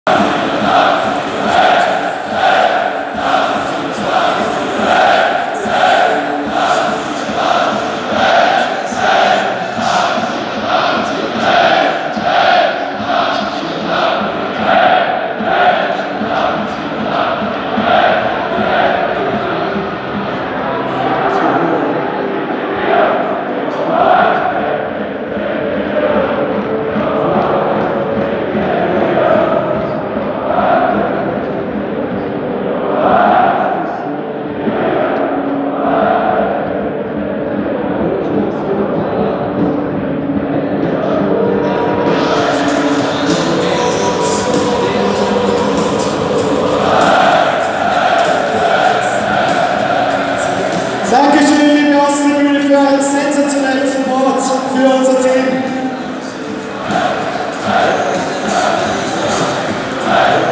Austria Wien Fans feiern den 6:1 Wiener Derby-Sieg gegen Rapid am 16. Dezember 2018
FK Austria Wien - SK Rapid Wien 6-1 Generali Arena